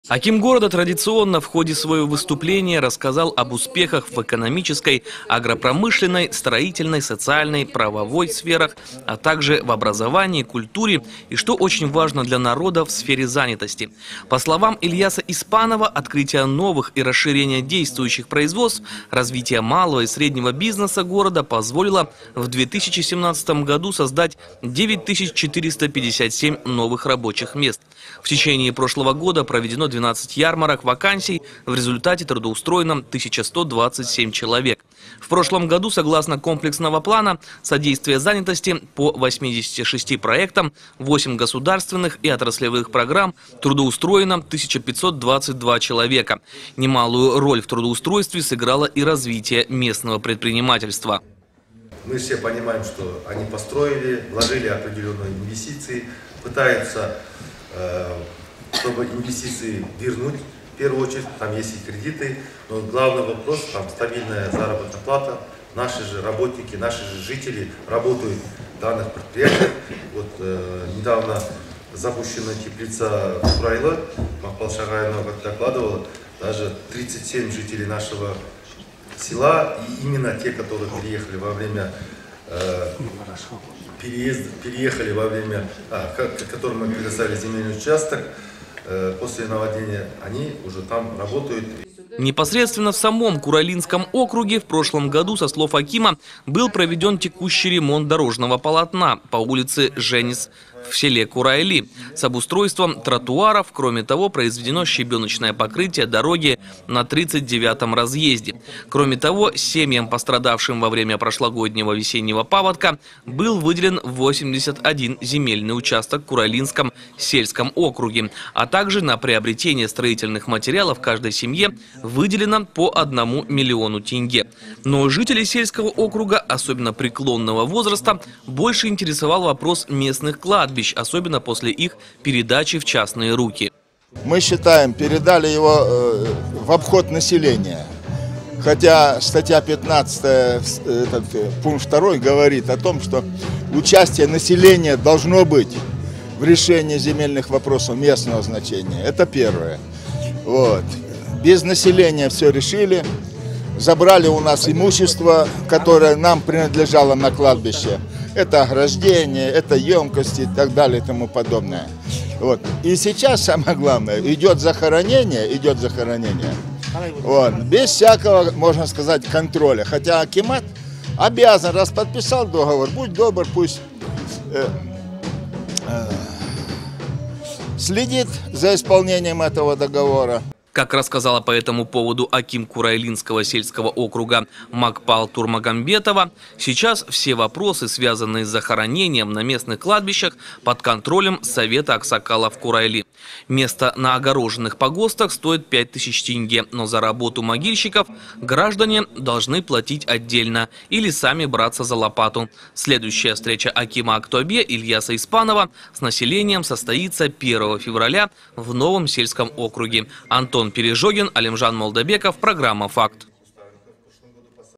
Аким города Ильяс Испанов провел отчетную встречу с жителями Курайлинского СО
Аким города Ильяс Испанов сегодня провел очередную отчетную встречу. На этот раз с населением Курайлинского сельского округа. В заседании также приняли участие инспектор администрации президента Казахстана Асхат Кайнарбеков, депутаты городского маслихата и члены общественного совета, а также руководство сельского округа. Аким города в ходе своего выступления, рассказал об успехах в экономической, агропромышленной, строительной, социальной, правовой сферах, а также в образовании, культуре, и что очень важно для народа - в сфере занятости. По словам Ильяса Испанова, открытие новых и расширение действующих производств, развитие малого и среднего бизнеса города позволило в 2017 году создать 9457 новых рабочих мест. В течение прошлого года проведено 12 ярмарок вакансий, в результате трудоустроено 1127 человек. В прошлом году, согласно комплексному плану содействия занятости, по 86 проектам 8 государственных и отраслевых программ, трудоустроено 1522 человек. Немалую роль в трудоустройстве сыграло и развитие предпринимательства. Непосредственно в самом курайлинском округе в прошлом году, со слов акима, был проведен текущий ремонт дорожного полотна по улице Женис в селе Курайли с обустройством тротуаров, кроме того, произведено щебеночное покрытие дороги на 39 разъезде. Кроме того, семьям, пострадавшим во время прошлогоднего весеннего паводка, был выделен 81 земельный участок в Курайлинском сельском округе. А также на приобретение строительных материалов каждой семье выделено по 1 млн. тенге. Но жителей сельского округа, особенно преклонного возраста, больше интересовал вопрос местных кладбищ, особенно после их передачи в частные руки. Как рассказала по этому поводу аким Курайлинского сельского округа Макпал Турмагамбетова, сейчас все вопросы, связанные с захоронением на местных кладбищах, под контролем совета аксакалов Курайли. Место на огороженных погостах стоит 5 тысяч тенге, но за работу могильщиков граждане должны платить отдельно или сами браться за лопату. Следующая встреча акима Актобе Ильяса Испанова с населением состоится 1 февраля в Новом сельском округе.